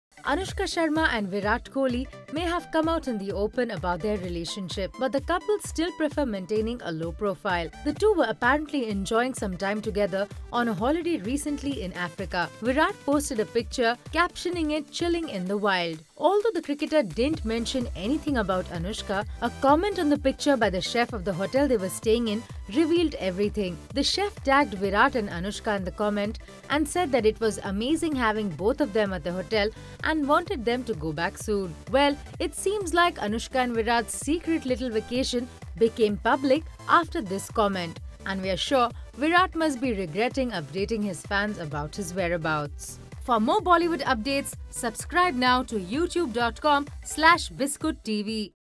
Anushka Sharma and Virat Kohli Exposed in Africa!
Anushka Sharma and Virat Kohli may have come out in the open about their relationship but the couple still prefer maintaining a low profile. The two were apparently enjoying some time together on a holiday recently in Africa.
Virat posted a picture captioning it Chilling in the wold. Although the cricketer didn't mention anything about Anushka, a comment on the picture by the chef of the hotel they were staying in revealed everything. The chef tagged Virat and Anushka in the comment and said that it was amazing having both of them at the hotel and wanted them to go back soon.

Well, it seems like Anushka and Virat's secret little vacation became public after this comment. And we're sure Virat must be regretting updating his fans about his whereabouts!

Watch latest bollywood gossip videos, bollywood news and behind the scene bollywood masala.